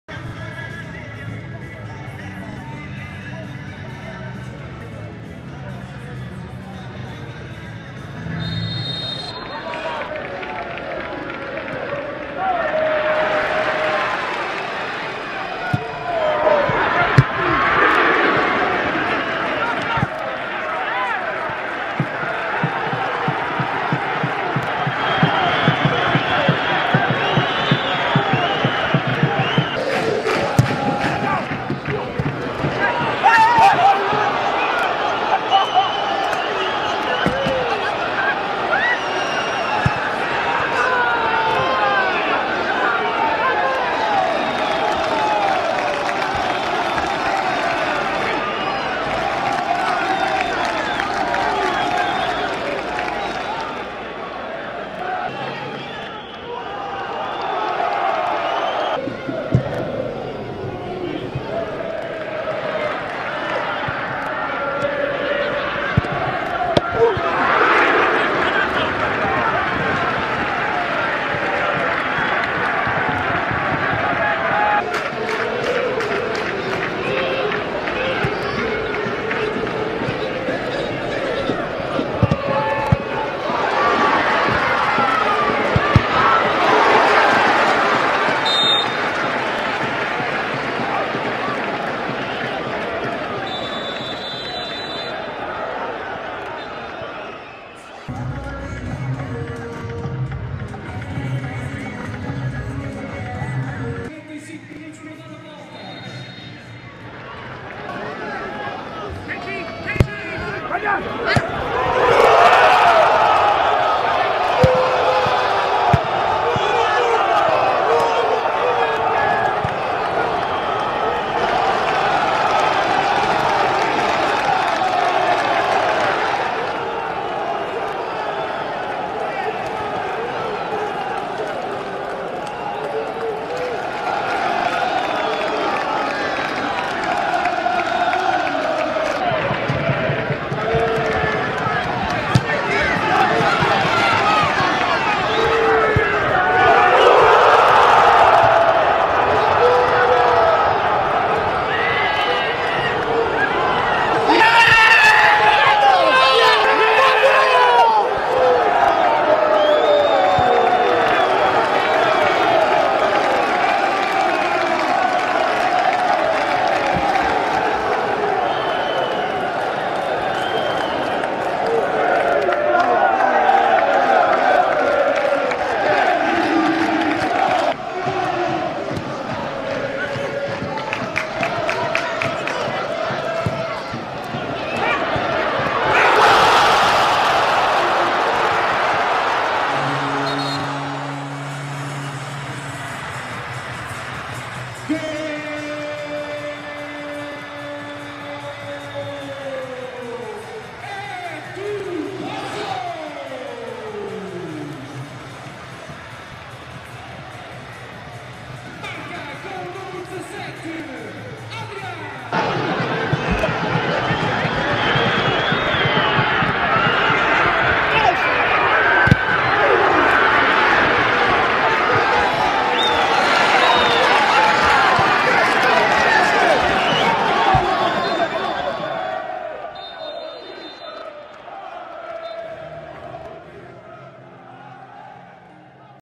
Highlights | Resumo: Paços de Ferreira 1-3 Boavista (Liga 22/23 #22)
Triunfo do Boavista por 3-1 no terreno do Paços de Ferreira, resultado que não permite ao conjunto pacense sair do último lugar da classificação da Liga Bwin. Já os axadrezados sobem ao 8.º lugar.